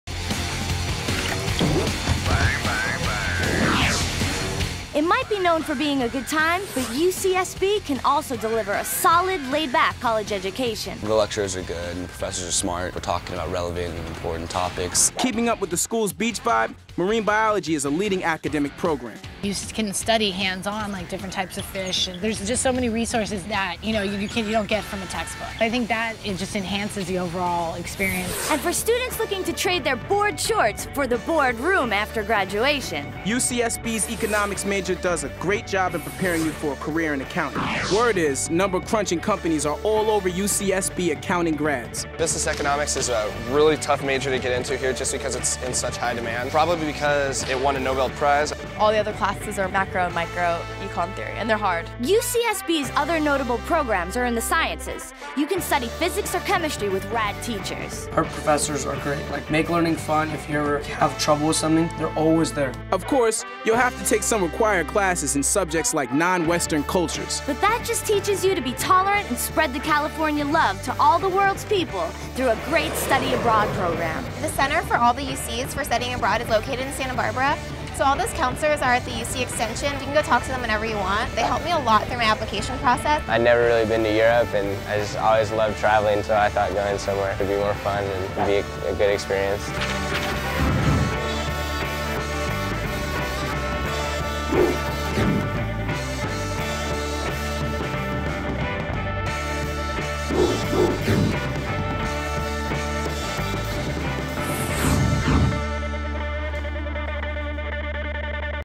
theU.com - UCSB: "Academics"
for 100,000 more college videos!

Students discuss the quality of Uc Santa Barbara's different academic departments, accessibility of professors/TA's, and level of workload. An original production from theU.com.